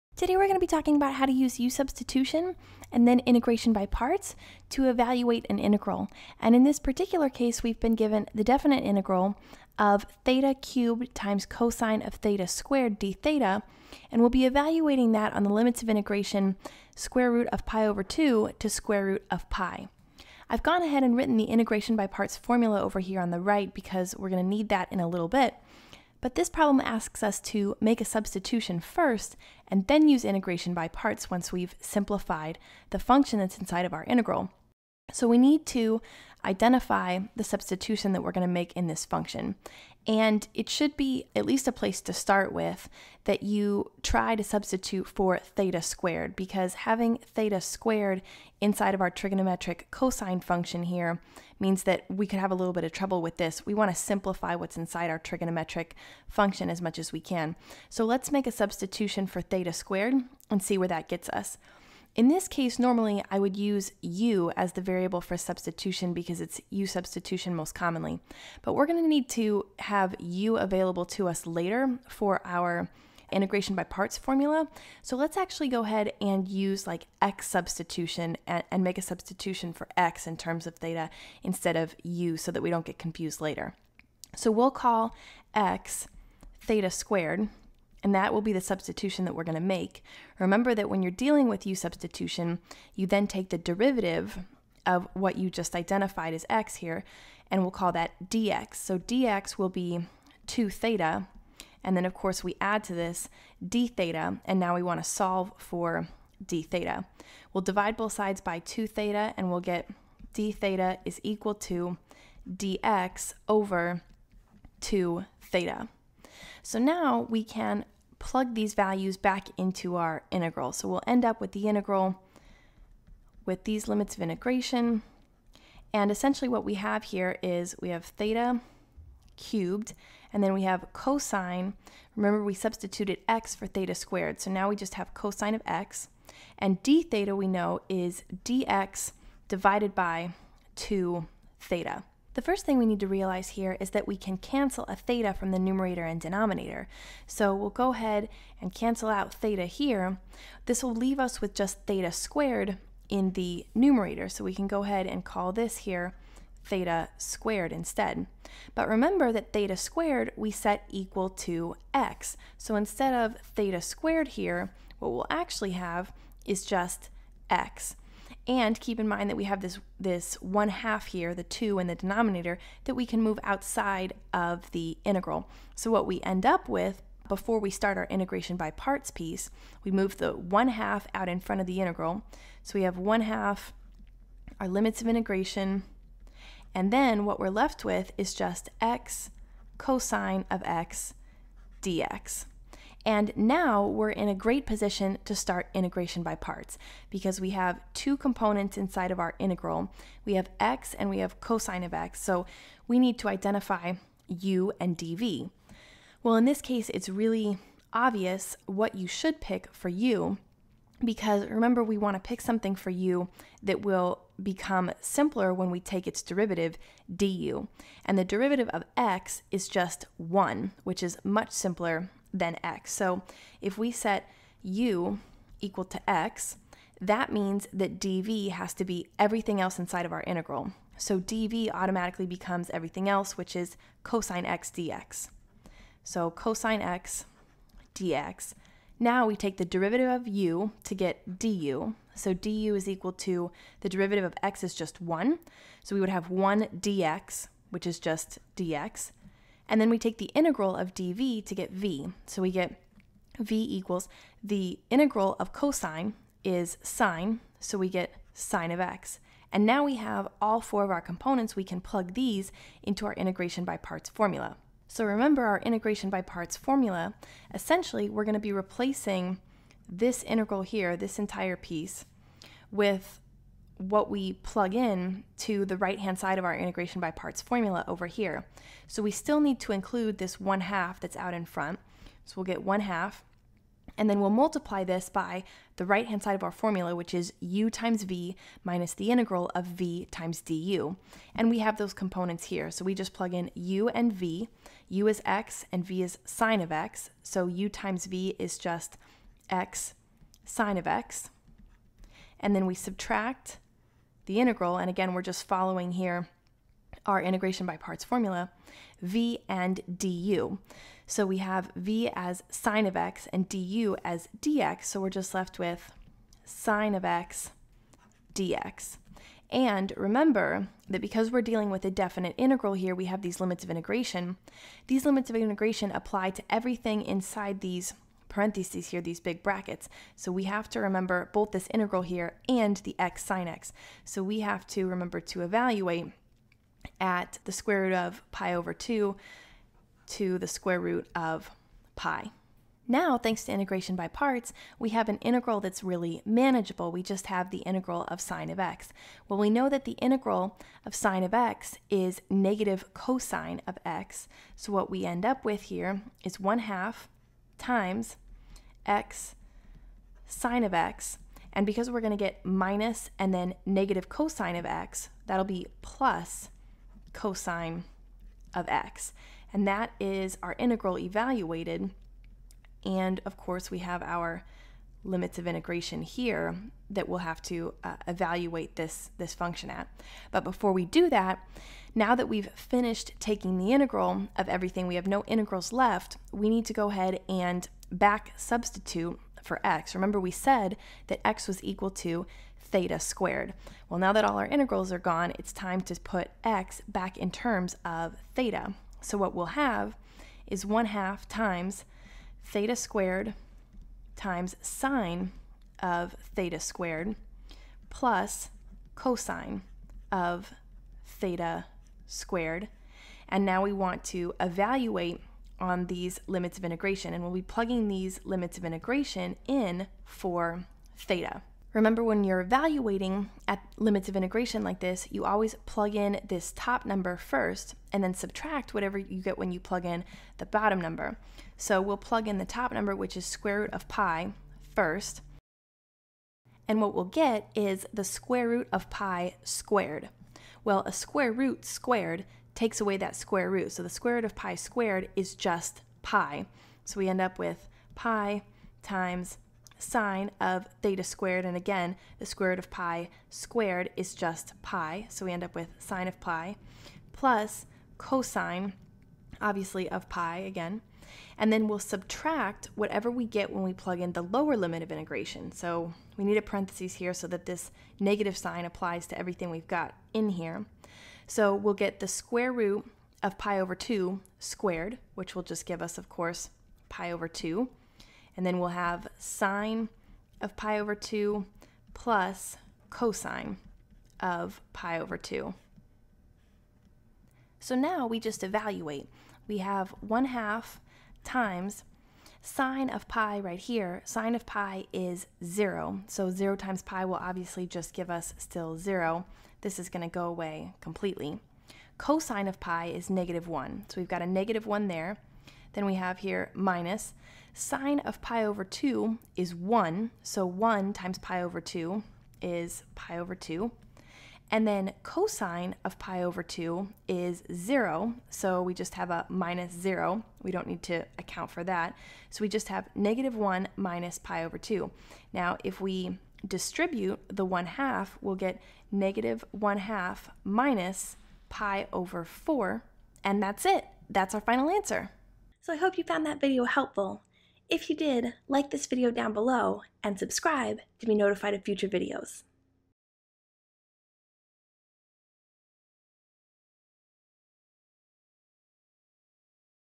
U-substitution with integration by parts (KristaKingMath)
Learn how to find the integral of a function using u-substitution and then integration by parts. Also, since this is a definite integral, evaluate at the limits of integration.